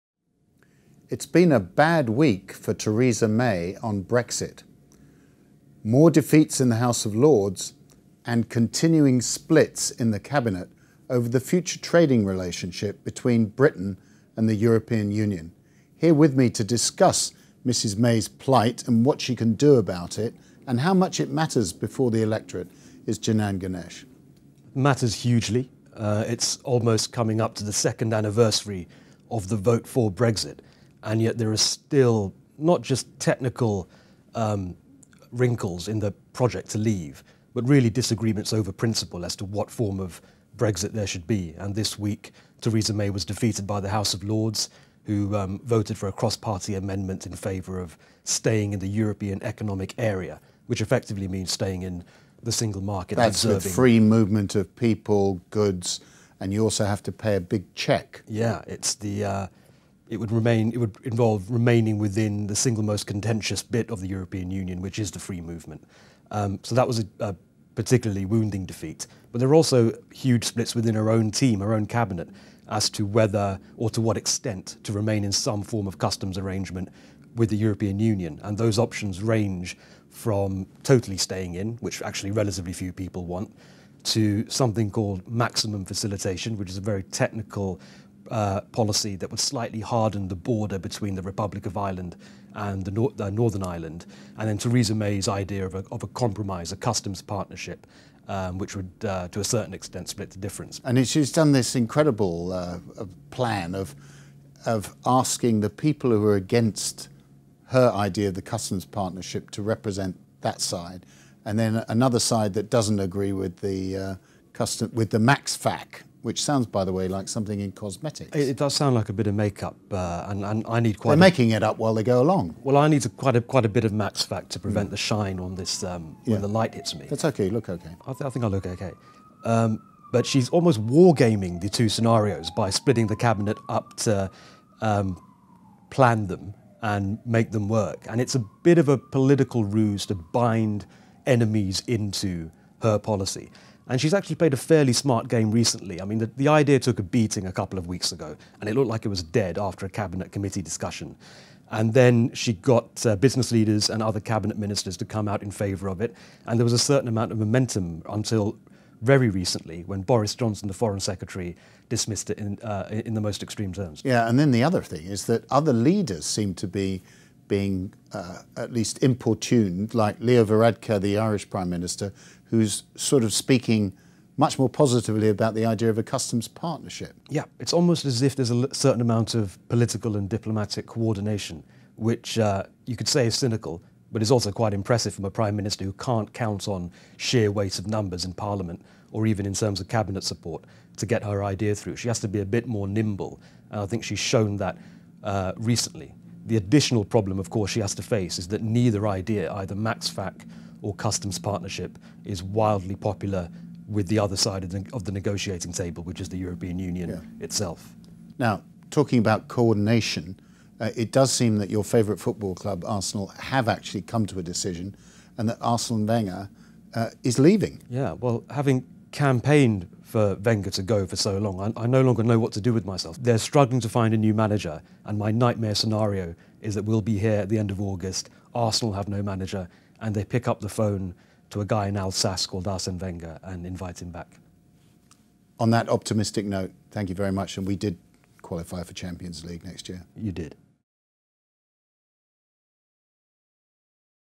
What Theresa May can do about her Brexit woes
FT editor Lionel Barber and political commentator Janan Ganesh discuss UK Prime Minister Theresa May's difficult week dealing with a warring Cabinet and opposition over Brexit in Parliament.